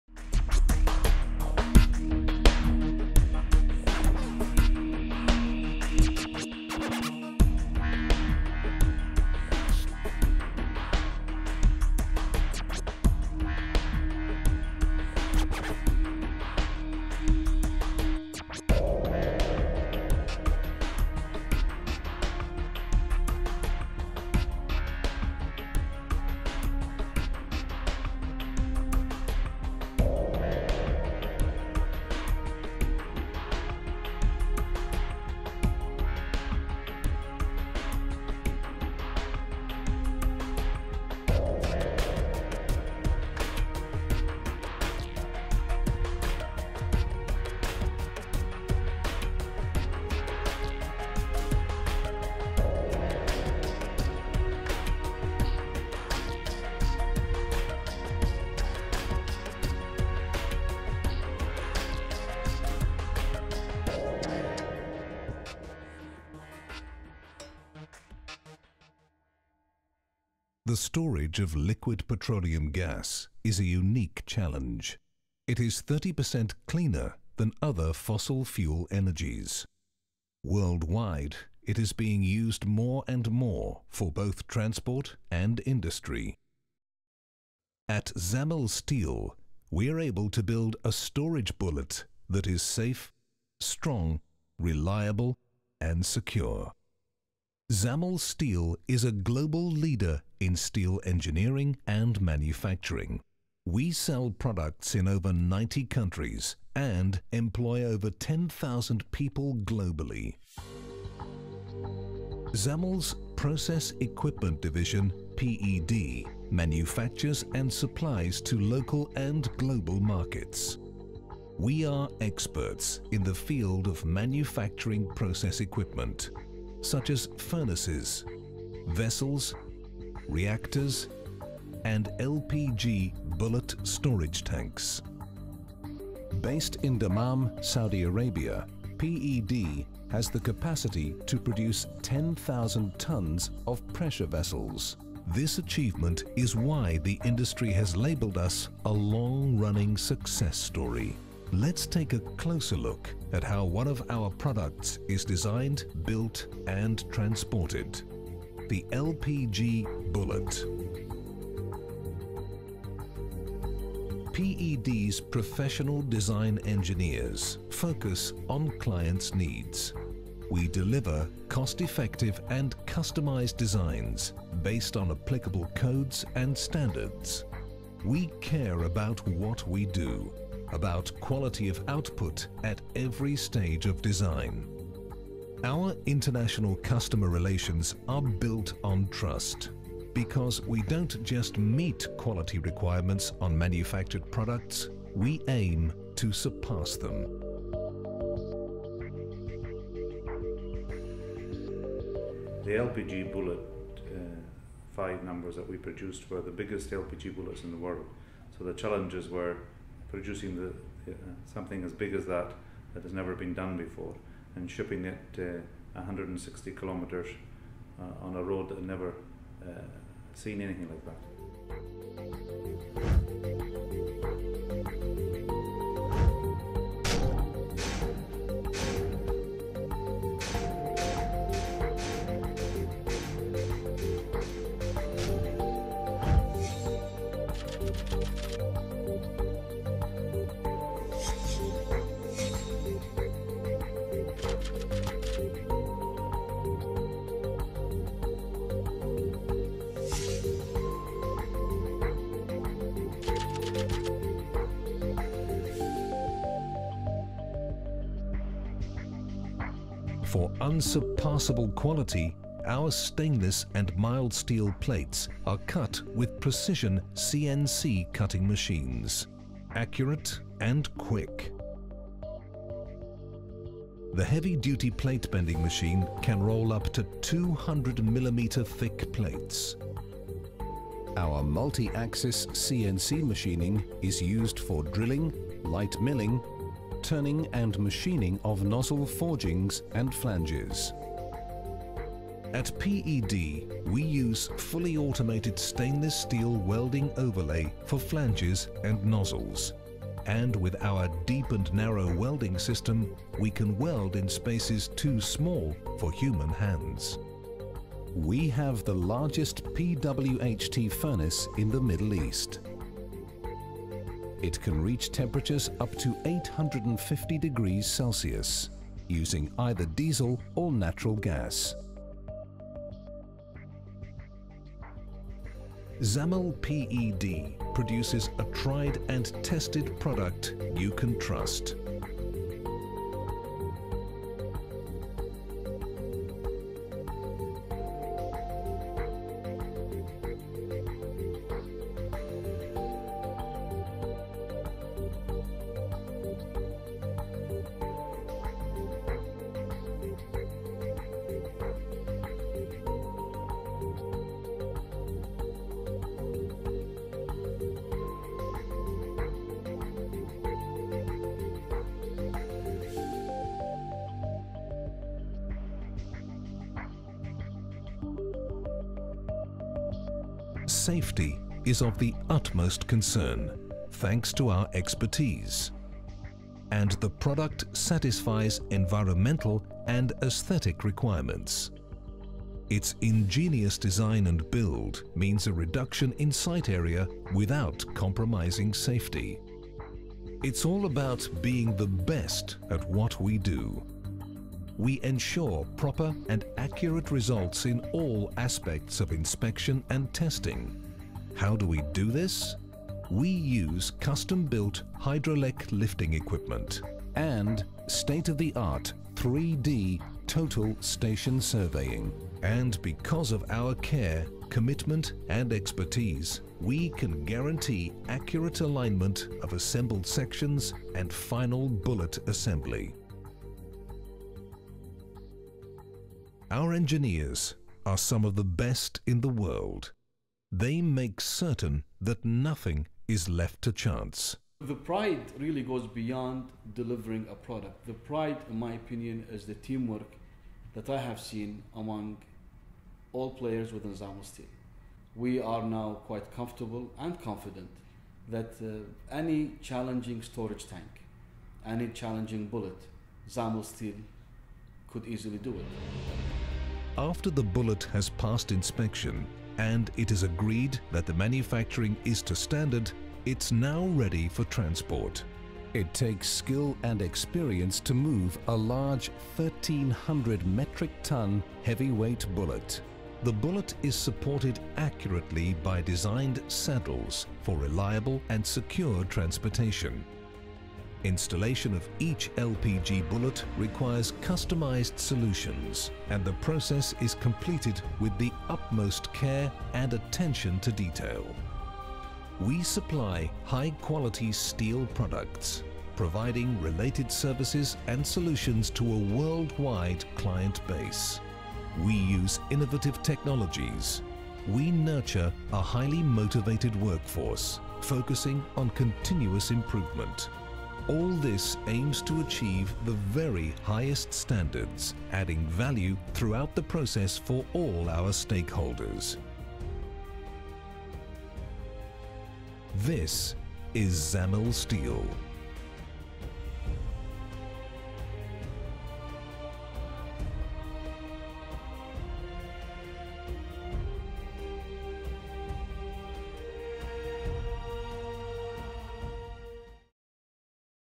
ZAMIL STEEL PED
The making of the largest LPG Storage Bullet
Produced by B FILMS
Directed by Bader Al Homoud and Bushra Al Eidy
انتاج مؤسسة بي فيلمز
اخراج بدر الحمود وبشرى العيدي